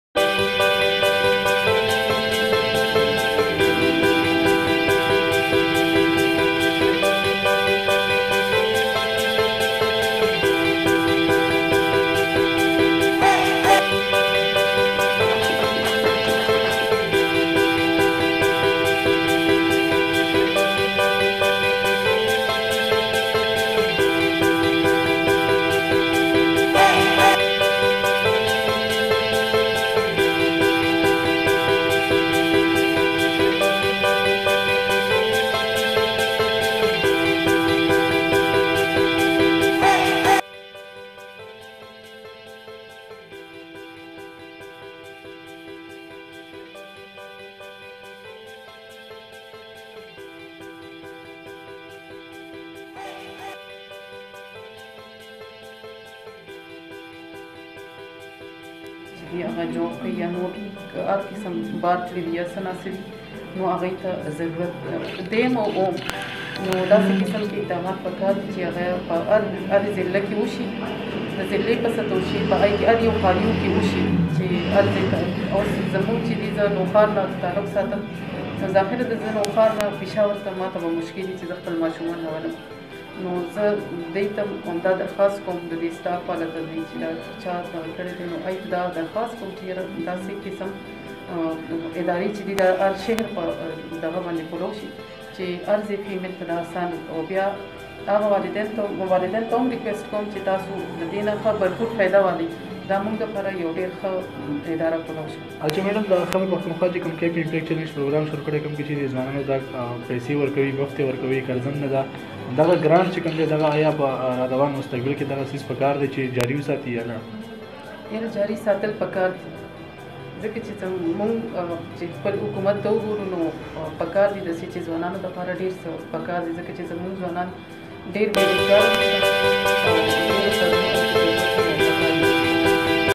Inauguration ceremony
Inauguration Ceremony of Daffodils
by honorable Chief guest Somi Falak Niaz(MPA)